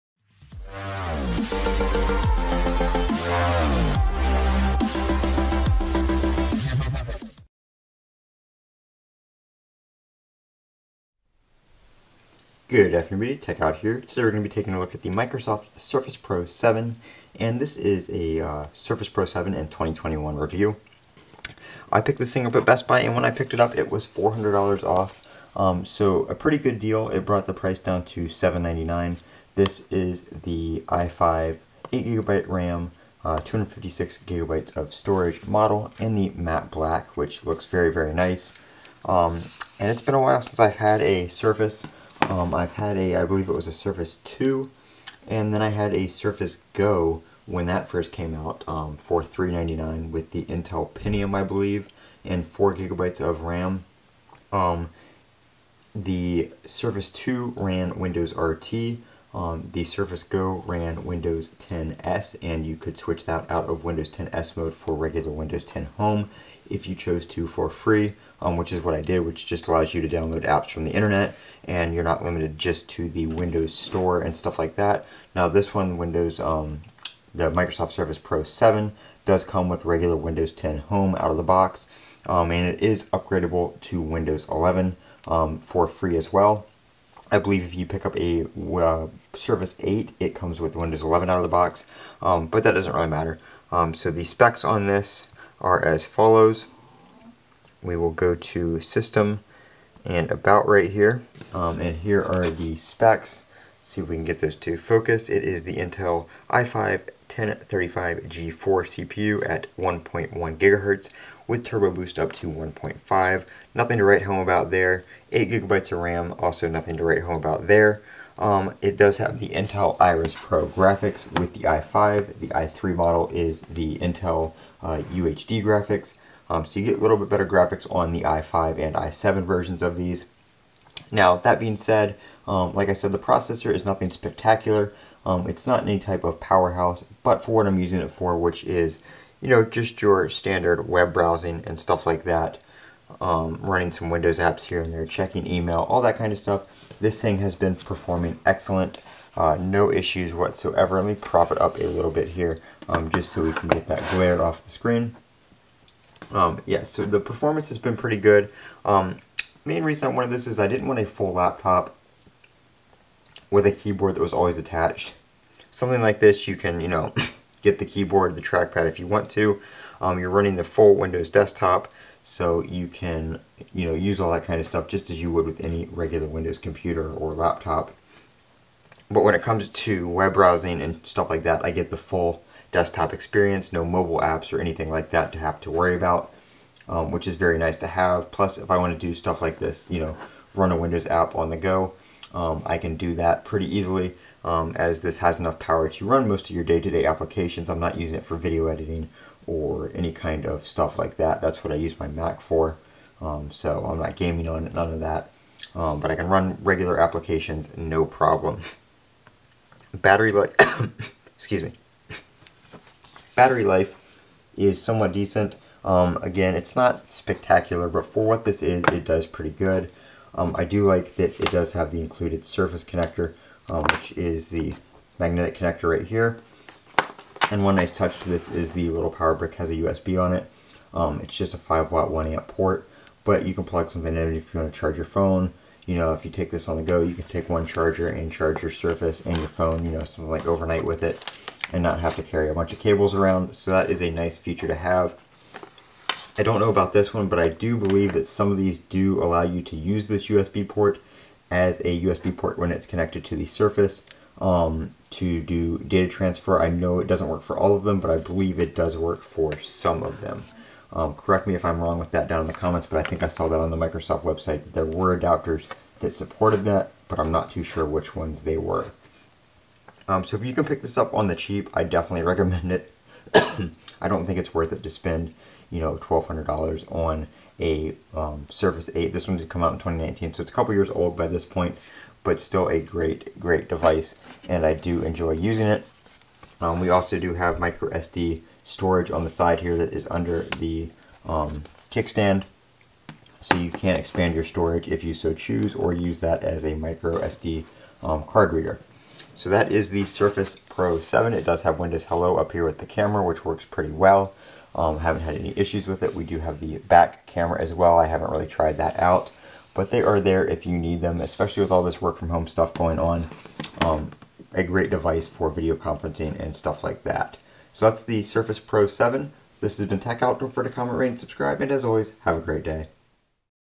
Surface Pro 7 With Windows 11 - Still Good In 2021?
Taking a look at the surface pro 7 from 2019 running newly released Windows 11. Does it still perform in 2021 with it's new OS?